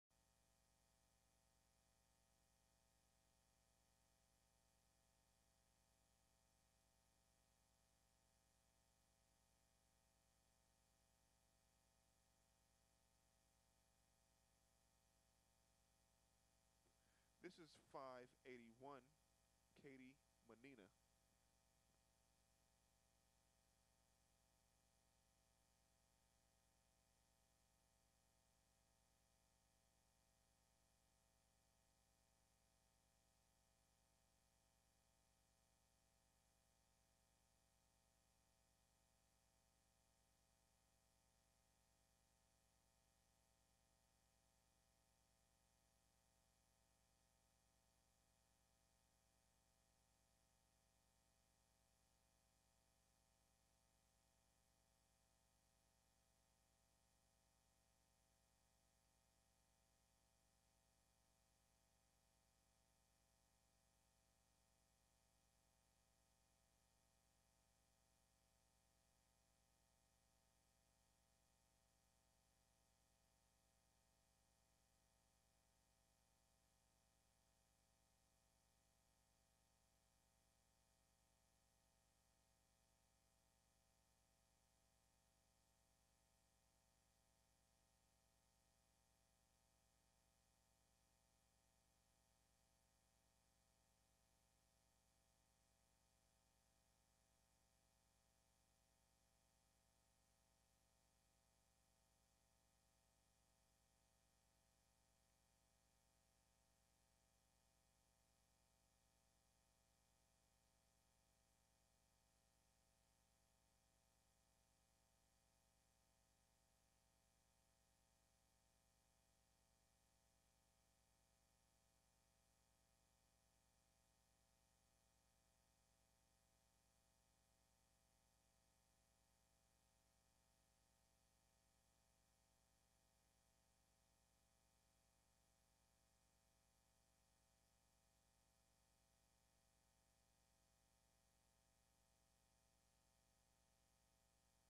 209 - Katie Mannina - Fortune Cookie
Amateur Hunt Seat Equitation Over Fences 18 - 35